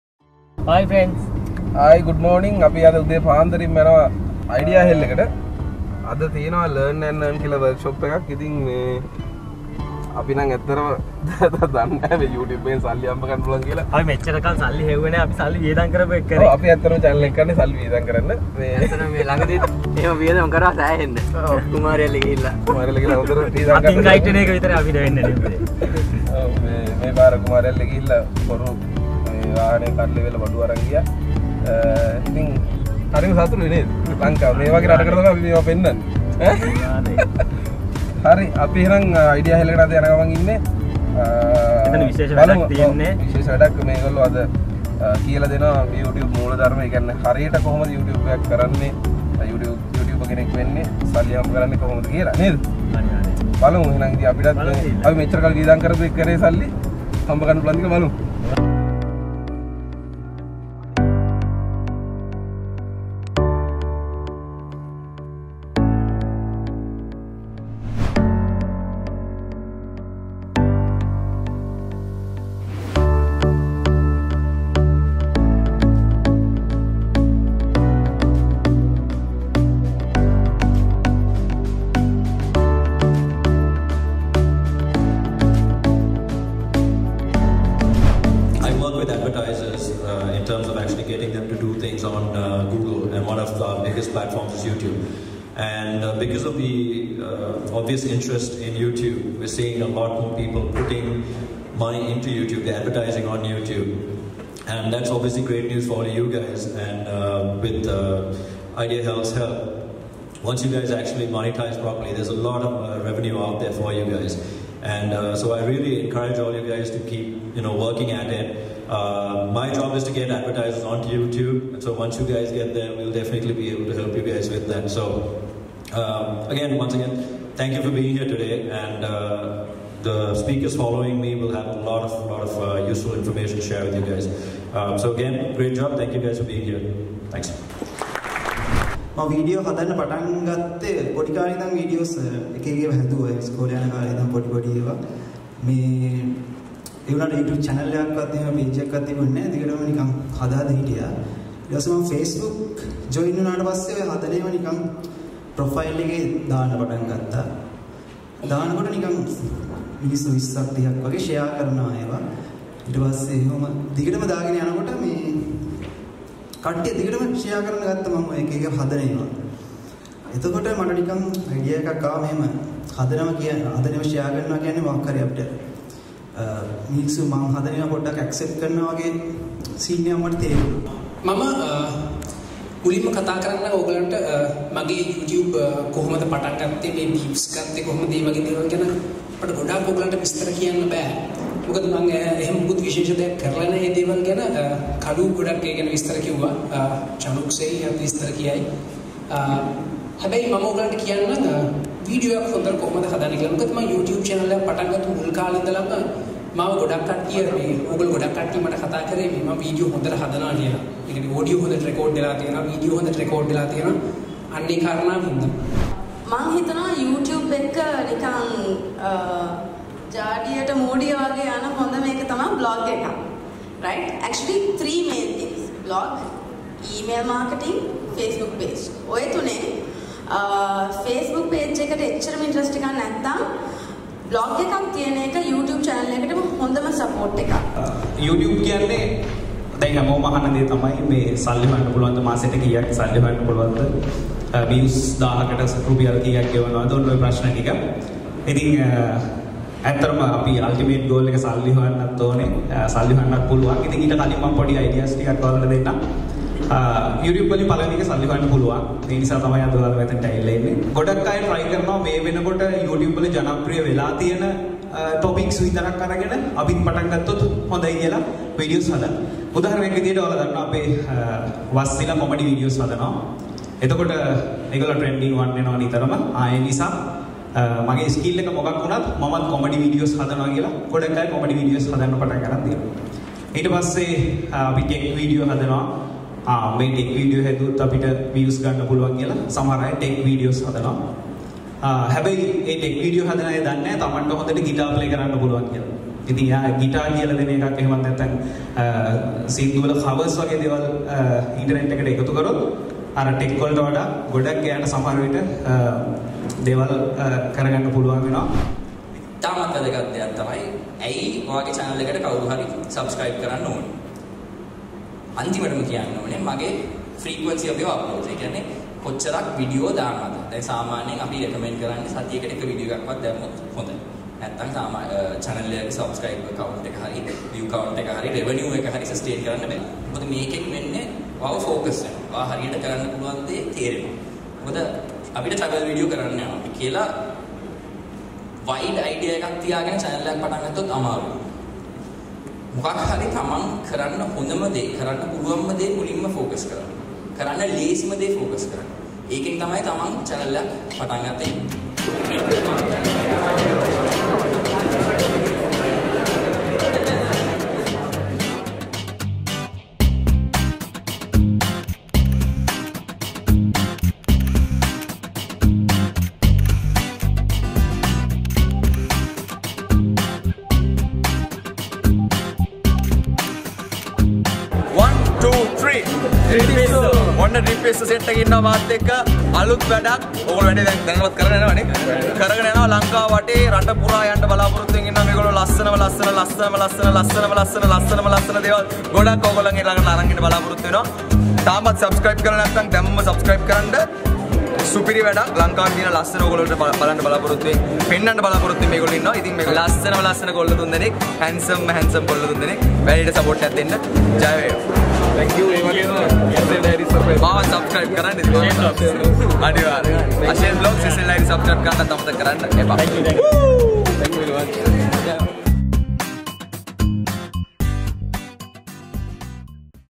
Learn and Earn Youtube Workshop - by IdeaHell TRIP PISSO
Learn & Earn Workshop :
Do you dream of being a YouTuber?
How do you start off creating a channel?
What are the ways of monetising on YouTube?
just join with IdeaHell :)